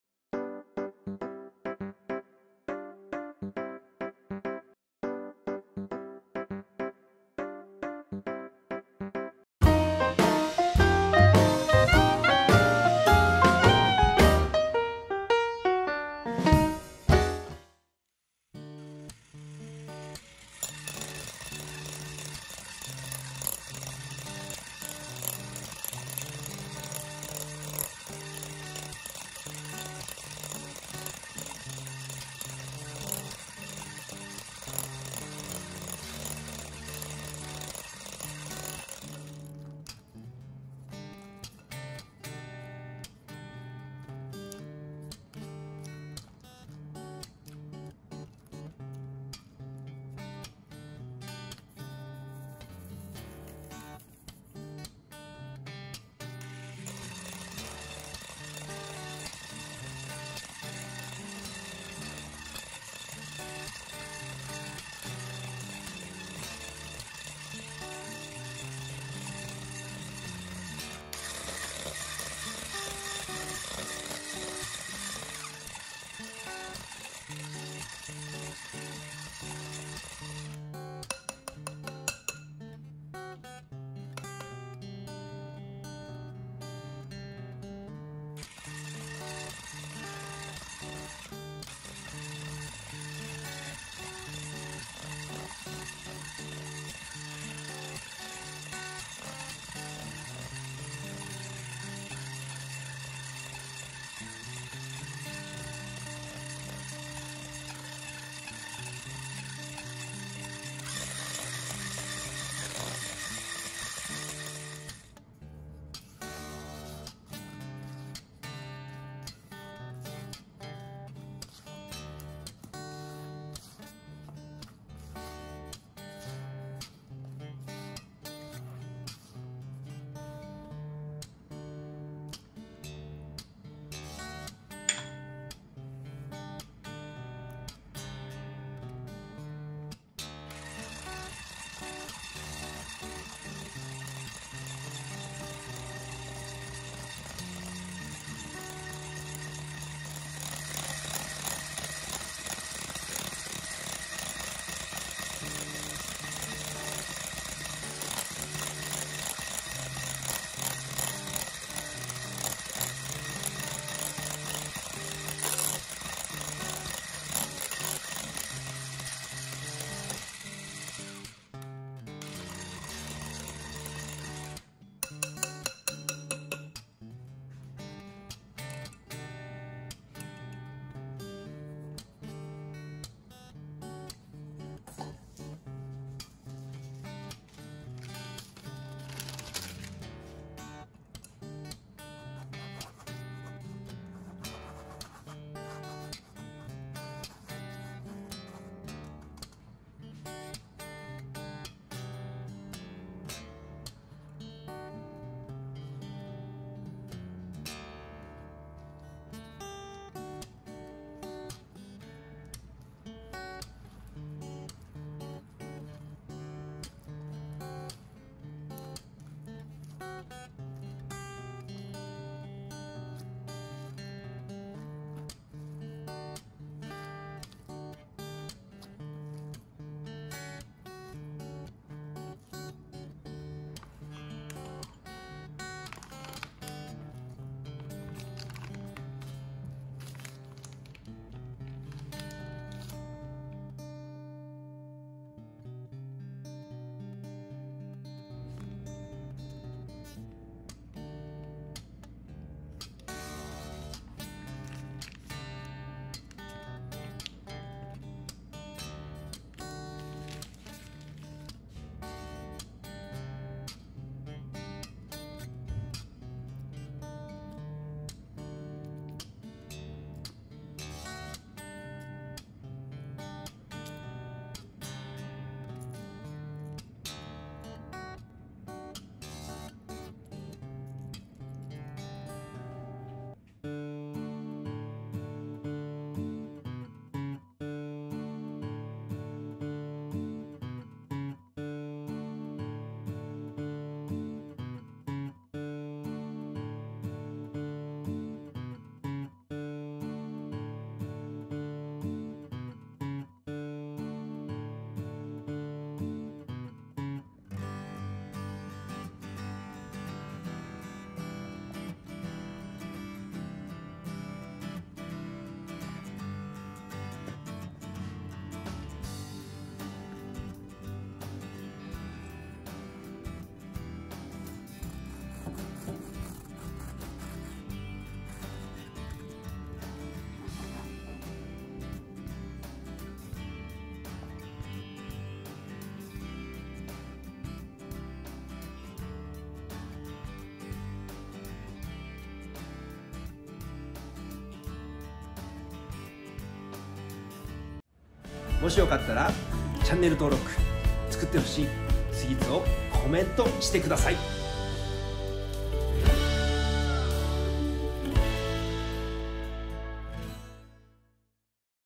【チーズ・マドレーヌ】パルメザンチーズを使ったマドレーヌの作り方&レシピ
【チーズインマドレーヌ】パルメザンチーズを使ったマドレーヌの作り方&レシピ

〜レシピ〜
バター　　　110g
全卵　　　　110g
上白糖　　　110g
パルメザン　40g
チーズ
薄力粉　　　110g
ベーキング
パウダー　　　2g
パルメザン       15g
チーズ(飾り)

お家でも作れるチーズインマドレーヌの作り方とレシピの紹介です。

市販の粉チーズを使ってマドレーヌを焼きました。

チーズの香りとバターの味がとても合うマドレーヌです。

 ぜひお時間あれば参考にしてお家で作ってください。